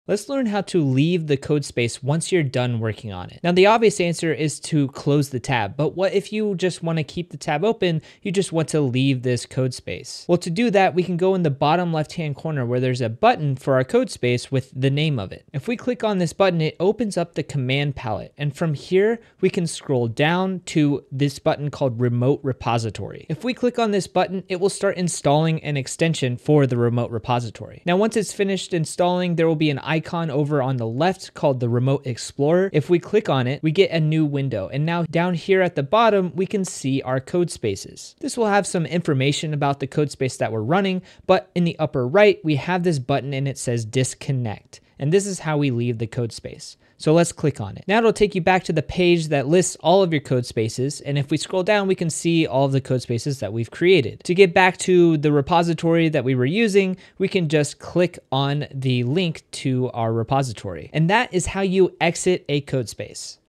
How to Exit a Codespace on Github
In this tutorial you'll learn how to exit or leave/close a codespace on Github without just closing the tab